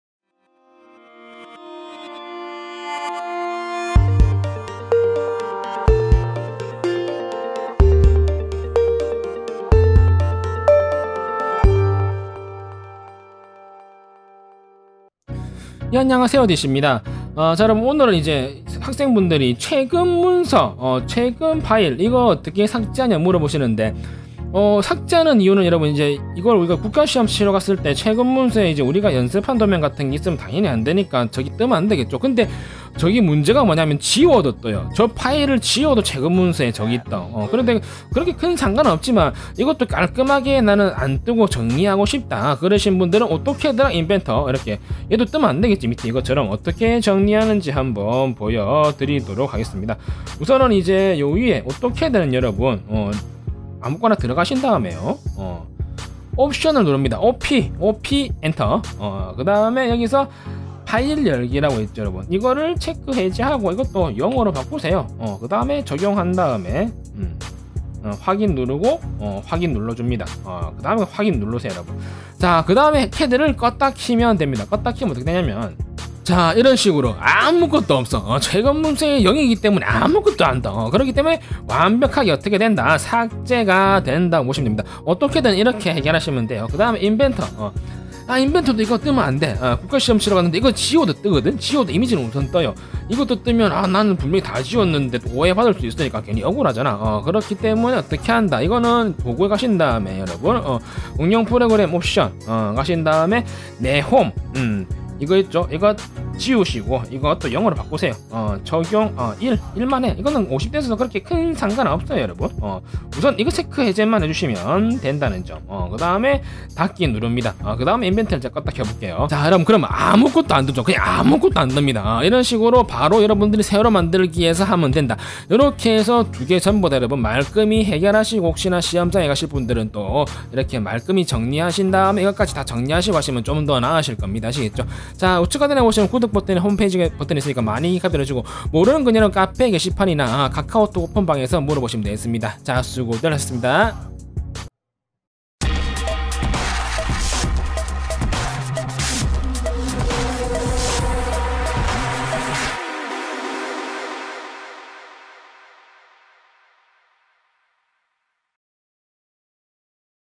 [기계설계산업기사 실기] 캐드&인벤터 실행시 "최근문서 삭제" 방법 (오토캐드 강좌)
👨‍👩‍👦 단체 카톡방